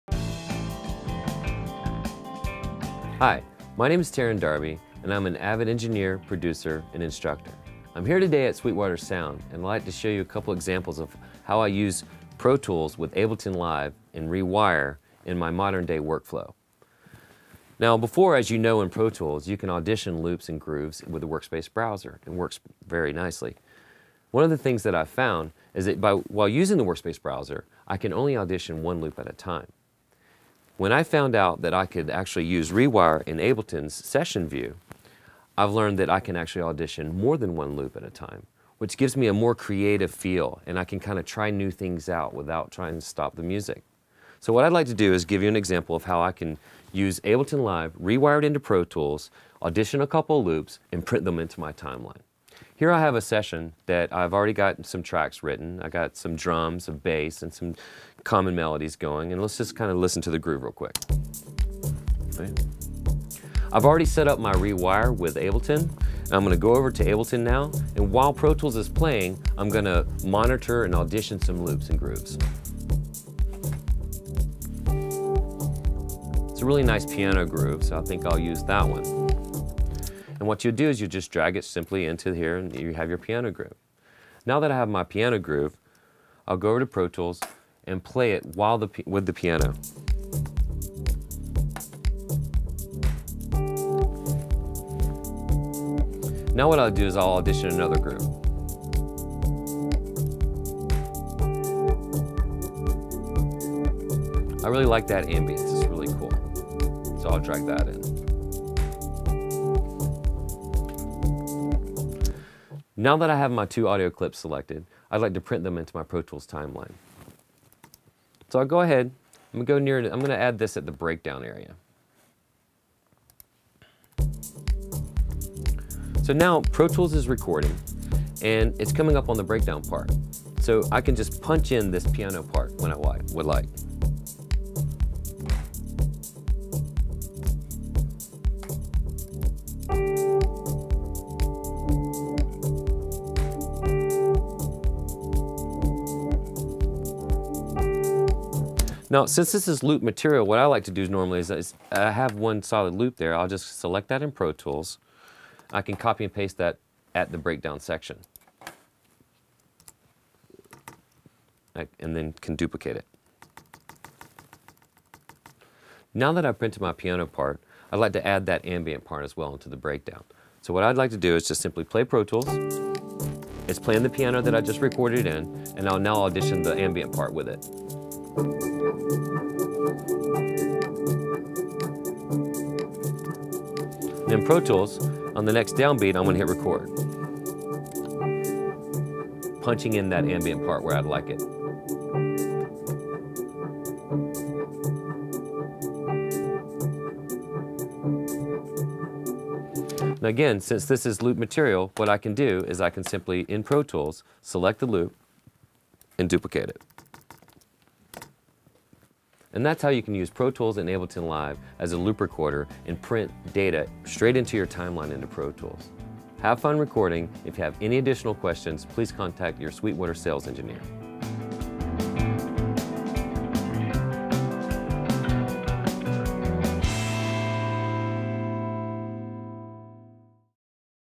Using Ableton Live with ProTools as a Loop Recorder (via ReWire)
Avid Engineer Terron Darby demonstrates how to use Pro Tools as a loop recorder for Ableton Live, using ReWire. In this example, Terron uses Live to audition different loops until he finds the right ones for his project. Then he records the loops into his Pro Tools session for arrangement and mixing.

While the Pro Tools Workspace Browser makes it easy to navigate your loop library, it only allows you to audition a single loop at a time. Using Ableton Live's Session view, you can audition multiple loops at a time, and switch between them on the fly while your Pro Tools project is playing. If you record and mix in Pro Tools, and also use lots of loops in your music, this is a great way to work.

By using ReWire to connect Ableton Live and Avid Pro Tools software, you're able to tap into the strengths of each program from the start of your project right through the finish.